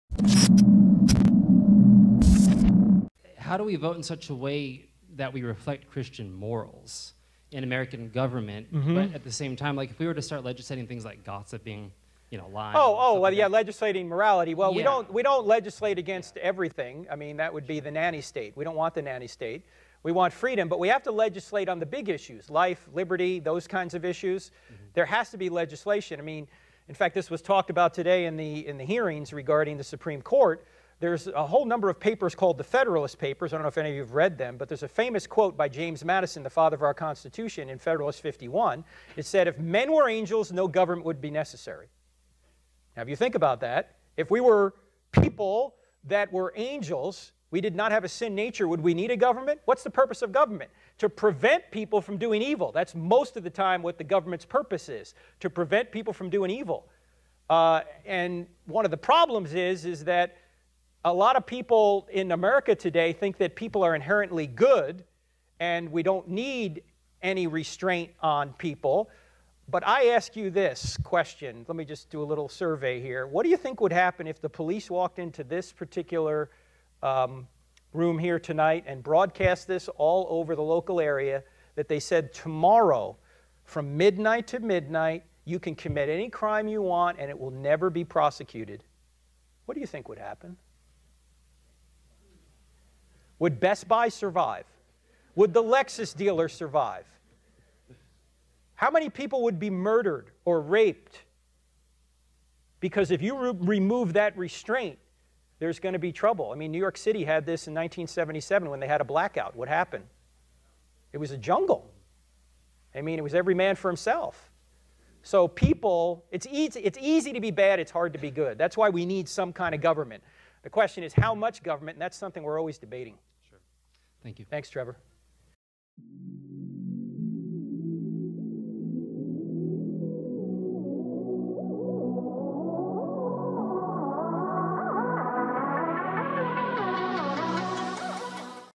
Are People Inherently Good?
Addressing some issues about legislating morality, government and human nature.